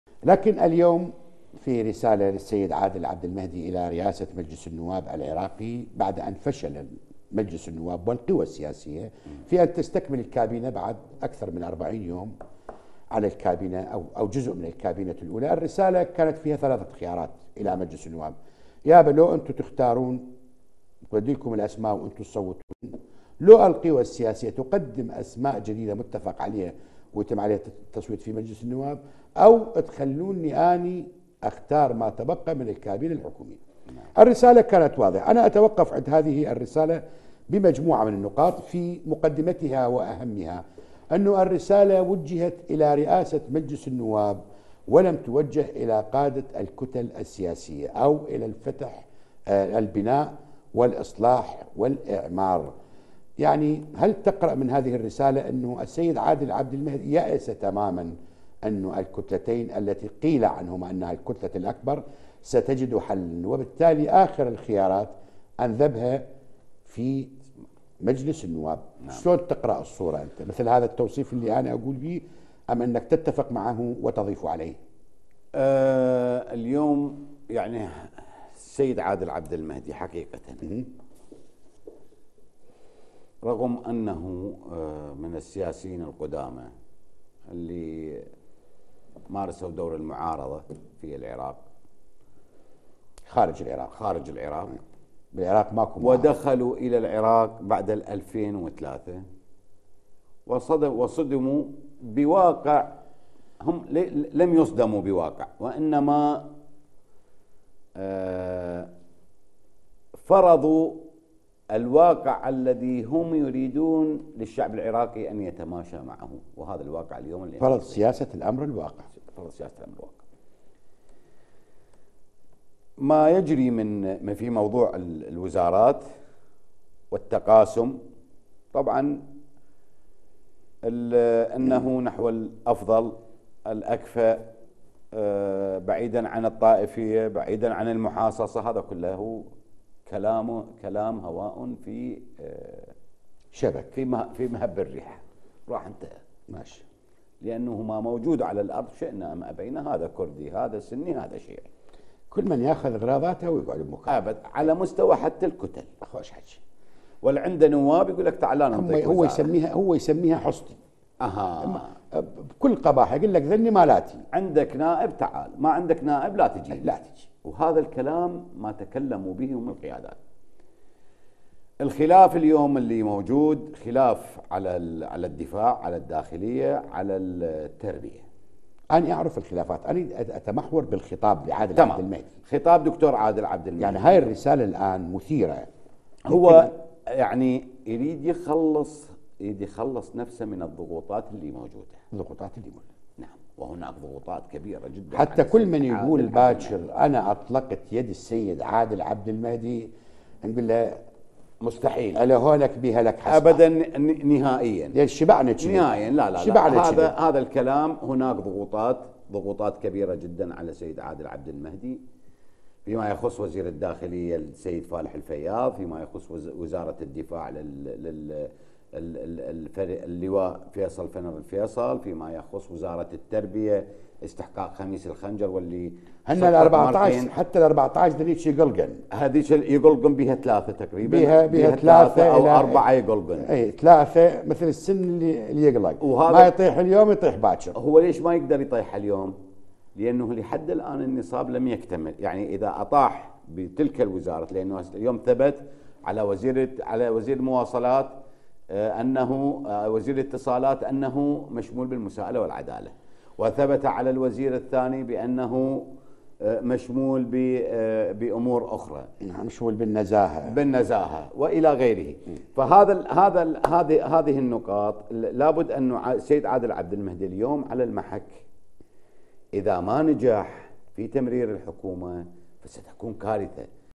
الشيخ شعلان الكريم: هناك ضغوطات كبيرة على عادل عبد المهدي في ما يخص اكمال الكابينة الوزارية
لقاء الاعلامي نجم الربيعي مع رئيس تحالف صلاح الدين الشيخ شعلان الكريم